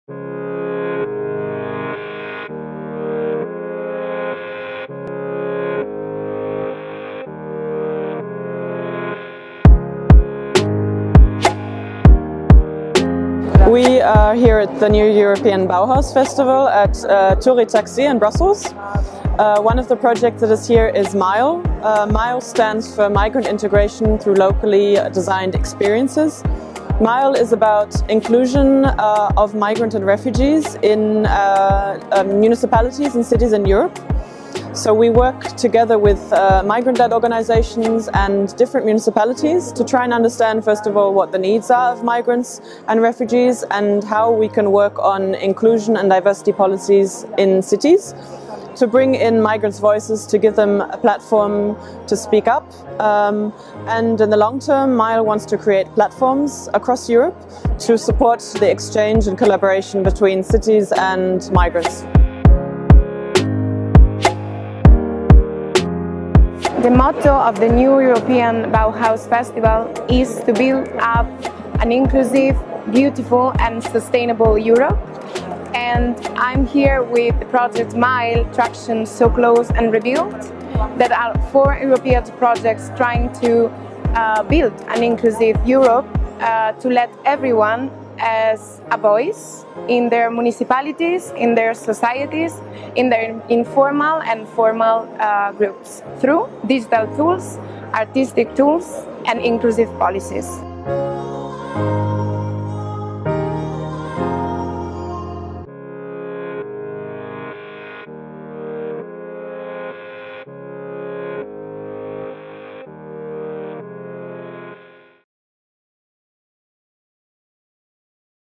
MILE project: What does migrant inclusion have to do with local democracy and a sustainable Europe?
MILE is a project about migrant inclusion and participation at the local level. We bring together different local migrant groups, municipalities and research teams across Europe to develop tools for diverse and inclusive policy-making.

This video has been filmed and produced by Umit Vurel.